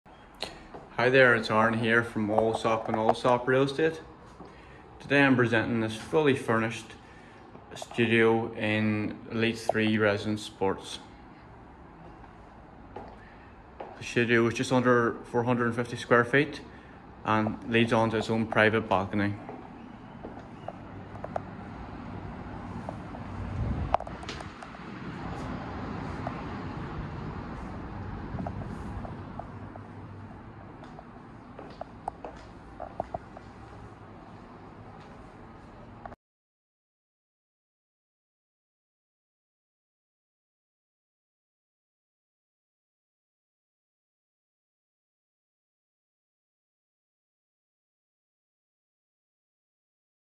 Spacious studio apartment with fantastic amenities in Dubai Sports City
Studio Apartment | Furnished | Vacant Now

Furnished Studio in Elite Sports Residence 3, Dubai Sports City is Vacant Now!

- Furnished Studio Apartment
- Fully Equipped Kitchen
- Spacious Balcony with Unobstructed View
- High Floor
- 444 Sq.ft
- Amenities Include Gym, Pool, Jacuzzi, Sauna and more!

This Fully Furnished Studio briefly comprises of a Spacious Entrance hall with Fully Equipped Kitchen and Ample Storage Space. The hall leads to a Well-Furnished Living Space with access to a Spacious Balcony. The property has a Well-Presented Bathroom as well as Large Built In Wardrobes. Tenants get to enjoy a variety of amenities which includes Communal Gym, Swimming Pool, Steam Room, Jacuzzi and more!